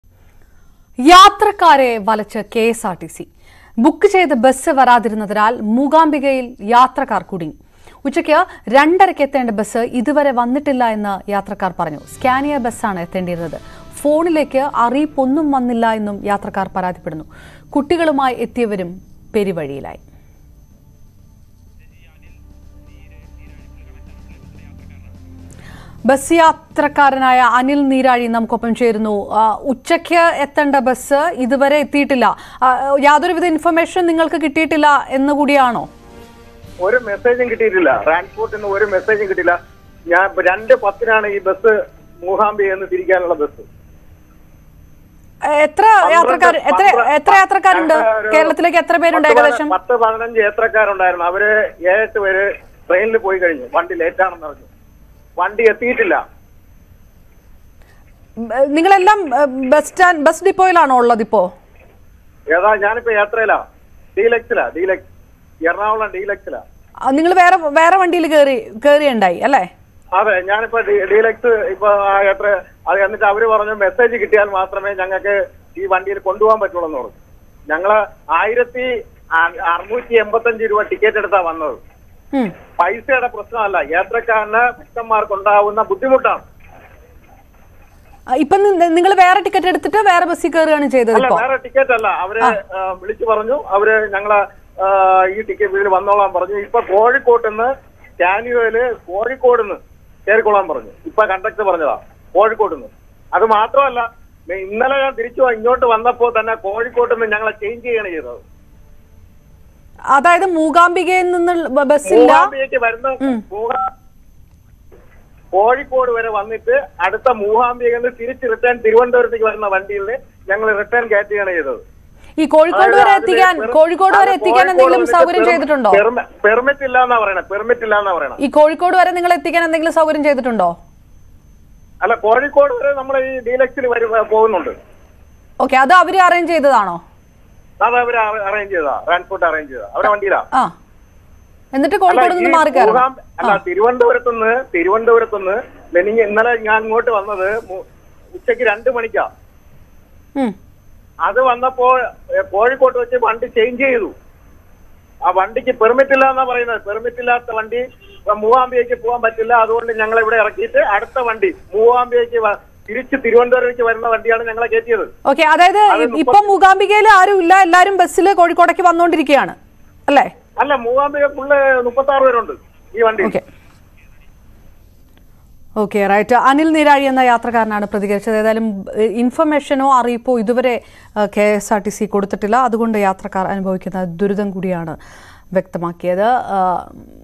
ബുക്ക് ചെയ്ത KSRTC Bus എത്തിയില്ല; യാത്രക്കാർ Mookambikaൽ കുടുങ്ങി | Karnataka | Malayalam News
യാത്രക്കാരെ വലച്ച്  KSRTC. Book ചെയ്ത ബസ് വരാതിരുന്നതിനാൽ Mookambikaയിൽ യാത്രക്കാർ കുടുങ്ങി. ഉച്ചയ്ക്ക്  2.30 എത്തേണ്ട ബസ് ഇതുവരെ വന്നില്ലെന്ന് യാത്രക്കാർ പറഞ്ഞു.  സ്കാനിയ  ബസ്സാണ് എത്തേണ്ടിയിരുന്നത്.  ഫോണിലേക്ക് അറിയിപ്പൊന്നും വന്നില്ല എന്നും യാത്രക്കാർ പരാതിപ്പെടുന്നു. കുട്ടികളുമായി എത്തിയവരും പെരുവഴിയിലായി.